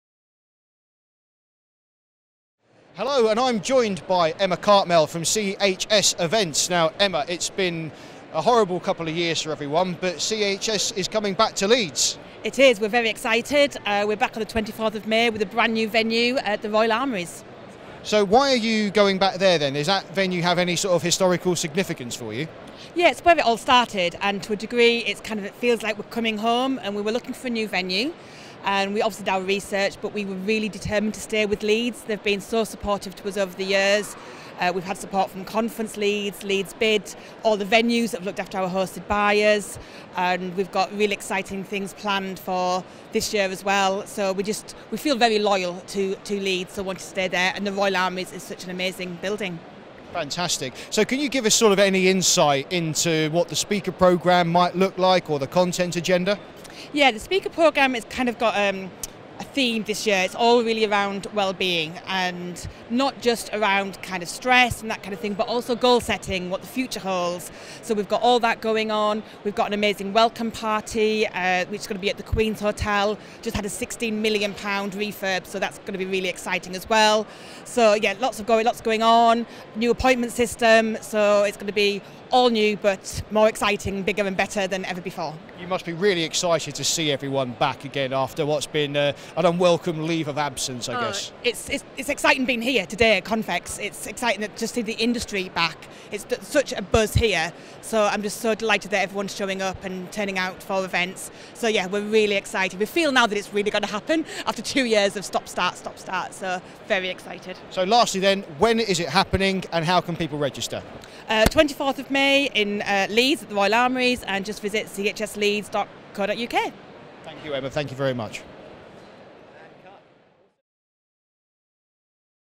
CN Interview at International CONFEX '22 with Emma Cartmell, CHS Group
Martin Fullard, Editorial Director at Mash Media interviews Emma Cartmell of CHS Group at the CN Stand during International CONFEX '22